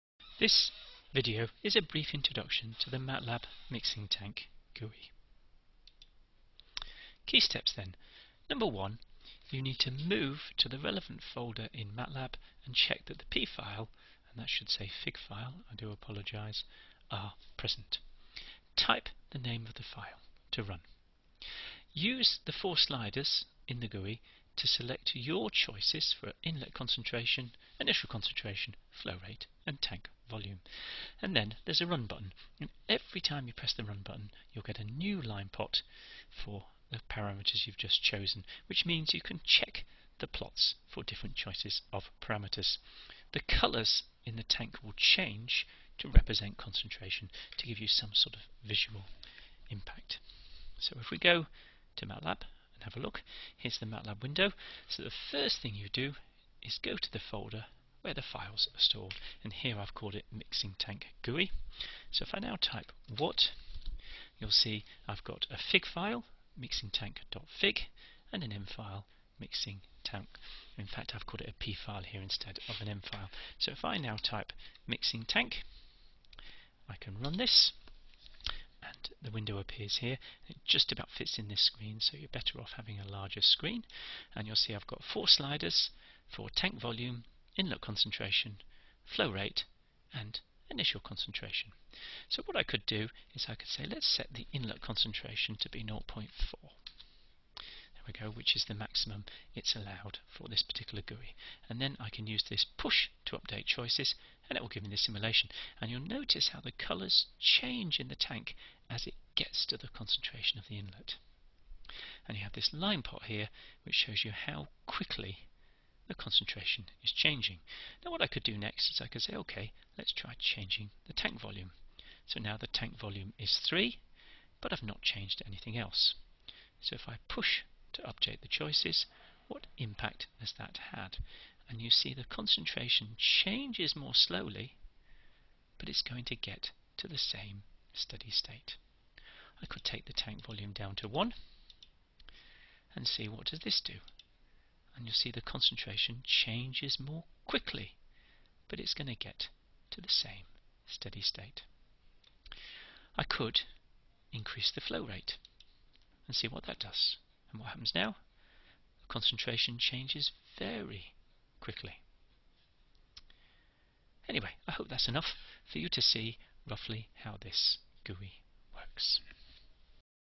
Mixing tank MATLAB GUI
Lectures aimed at engineering undergraduates. Presentation focuses on understanding key prinicples, processes and problem solving rather than mathematical rigour.

Short video showing how to run the mixing tank GUI on MATLAB.